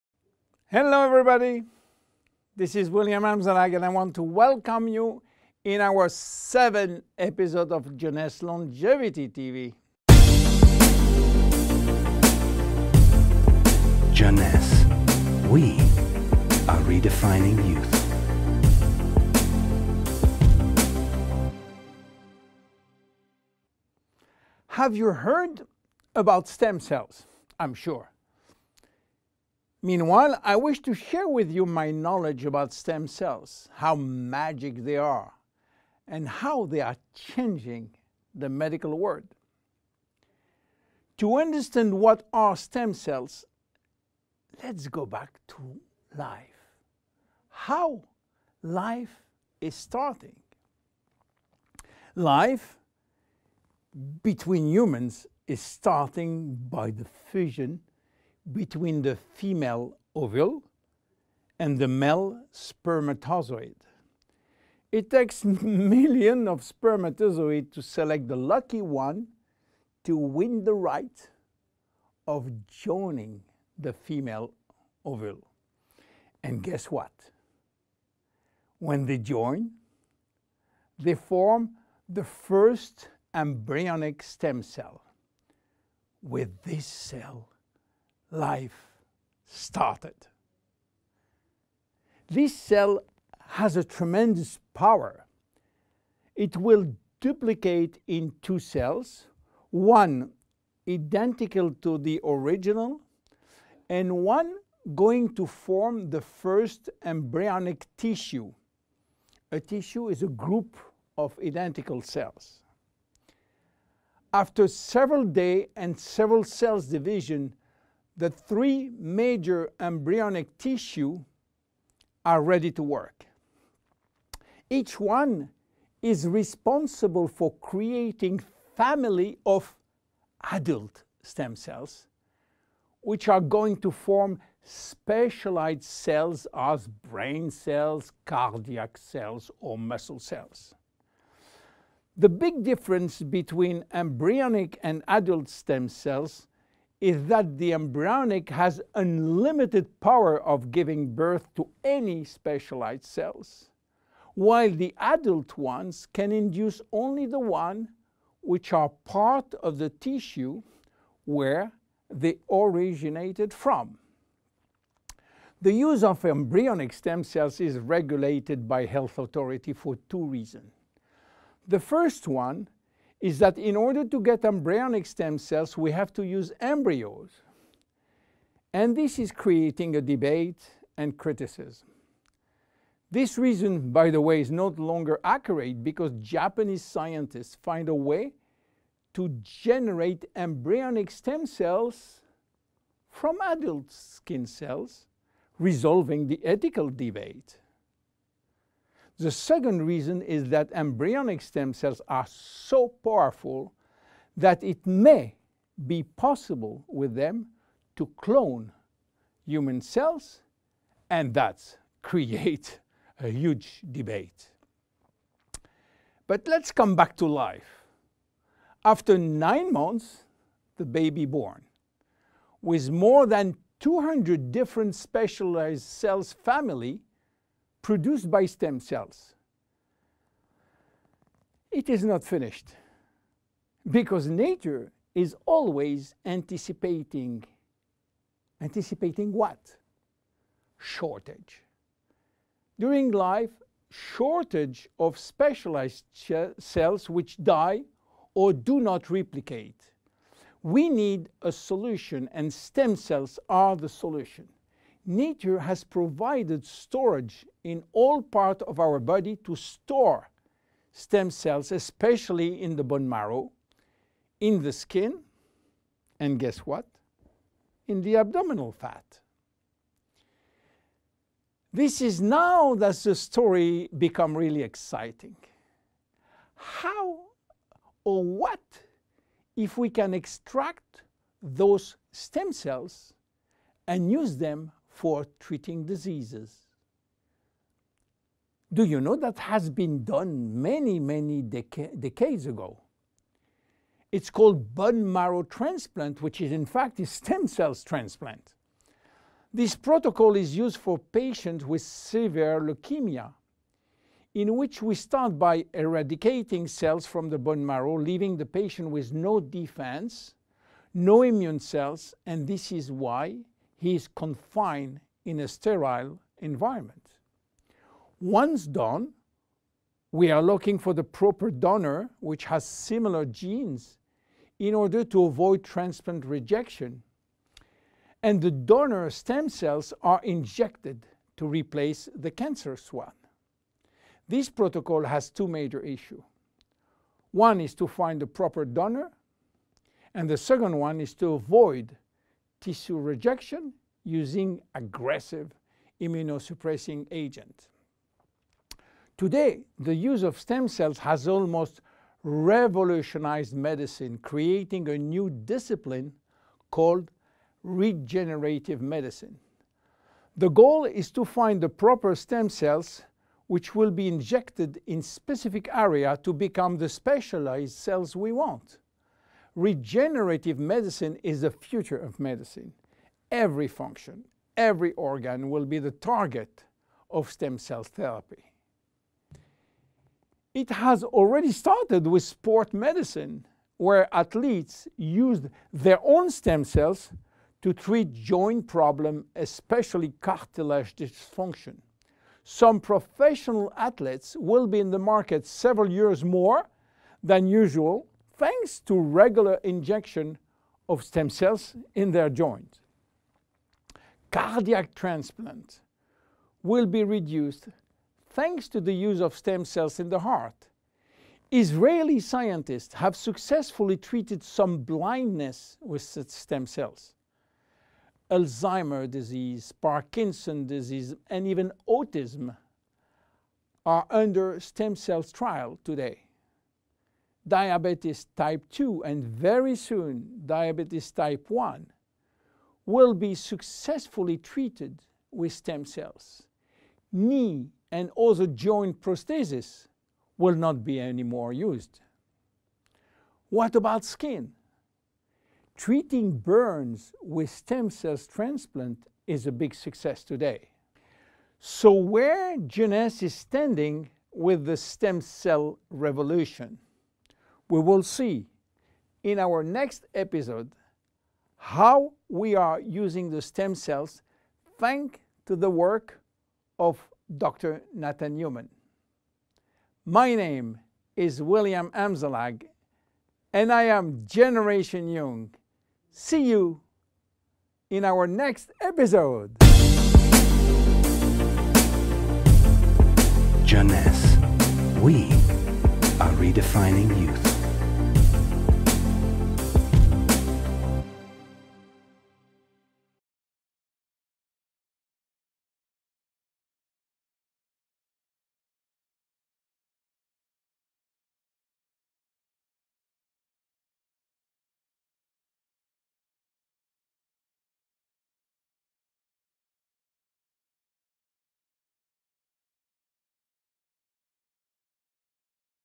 Jeunesse Longevity TV - Episodio 7 (Español)
EPISODIO 7: Entendiendo sobre Células Madre

Bienvenido a nuestro tercer episodio de Jeunesse Longevity TV - Generation Young, por Dr. William Amzallag, Director de Ciencias de los Productos y miembro del Consejo Médico de Jeunesse.

Subtítulos: Marcelo Ramos (Zotak7)